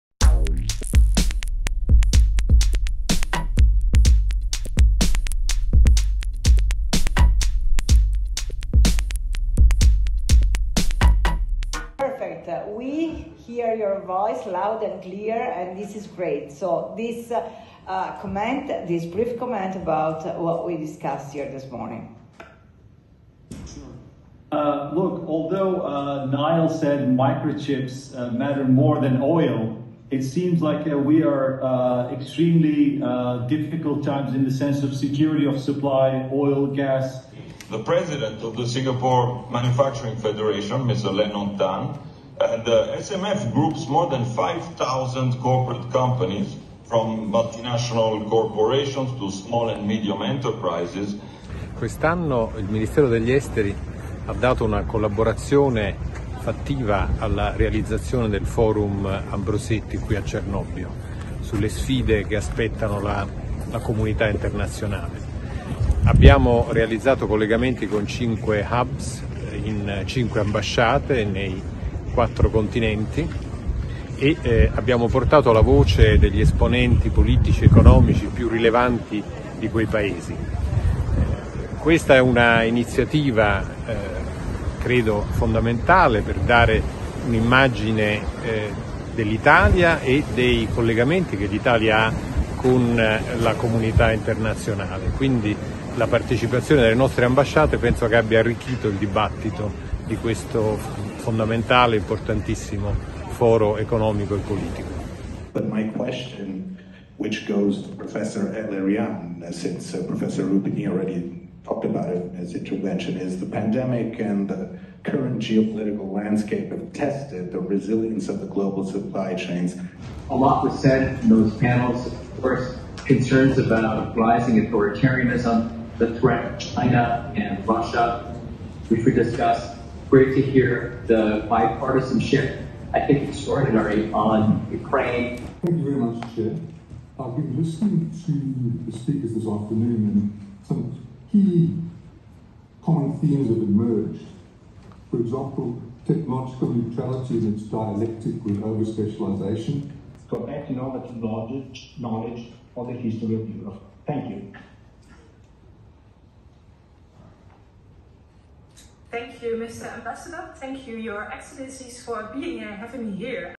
La Farnesina alla 48esima edizione del Forum di Cernobbio
Dal 2 al 4 settembre si è svolto, nella tradizionale cornice di Villa d’Este a Cernobbio, il Forum Ambrosetti “Lo Scenario di oggi e di domani per le strategie competitive” in un formato ibrido che ha consentito il collegamento da diversi hub di una eterogena e qualificata platea.

L’edizione 2022 del Forum di Cernobbio ha offerto la cornice per una collaborazione rafforzata tra il Ministero degli Affari Esteri e della Cooperazione Internazionale e the European House-Ambrosetti. I lavori del Forum hanno, infatti, potuto beneficiare del pieno coinvolgimento delle Ambasciate, che hanno arricchito la discussione con interventi di rappresentanti istituzionali, accademici e del mondo dei think tank dei rispettivi Paesi di accreditamento.

Il Ministro degli Affari Esteri e della Cooperazione Internazionale, Luigi Di Maio, è intervenuto alla sessione inaugurale nel panel dedicato alle sfide globali e al loro impatto sull’economia. Nel suo intervento, il Ministro ha illustrato l’impegno messo in campo dall’Italia, insieme ai suoi partner internazionali, per far fronte alle crisi che caratterizzano l’attuale scenario geopolitico, a cominciare dalle conseguenze dell’aggressione russa contro l’Ucraina, e le iniziative intraprese dalla Farnesina per far fronte alla difficile congiuntura internazionale.

Sono intervenuti al panel anche il Presidente ucraino Volodymyr Zelensky, in video collegamento; il Presidente dell’Azerbaijan, Ilham Aliyev, e la Ministra per le Relazioni Internazionali e la Cooperazione del Sud Africa, Naledi Pandor.

La sessione di apertura ha visto poi la partecipazione dall’Ambasciata d’Italia ad Ankara del Vice Ministro turco per l’Energia, Alparslan Bayraktar, e dall’Ambasciata a Singapore del Presidente della Singapore Manufacturing Federation, Lennon Tan.  A seguire, l’Ambasciata a Pretoria ha fornito spunti di riflessione sui temi scientifici grazie al contributo del Managing Director del South African Radio Astronomy Observatory, Rob Adam. In collegamento dall’Ambasciata a Washington, una qualificata rappresentanza di think tank americani e di funzionari del Dipartimento di Stato ha partecipato alla sessione dedicata agli USA. Nella giornata del 3 settembre, riservata ai temi europei, l’Ambasciata a L’Aja ha avuto modo di stimolare la discussione sulle questioni comunitarie attraverso l’intervento della politologa Beatrice de Graaf. L’Ambasciata d’Italia a Pechino ha organizzato il “ChinaHUB”, a cui hanno preso l’Ambasciatore americano in Cina, Nicholas Burns, e il Presidente della Camera di Commercio dell’Unione Europea in Cina e Presidente di BASF China, Joerg Wuttke.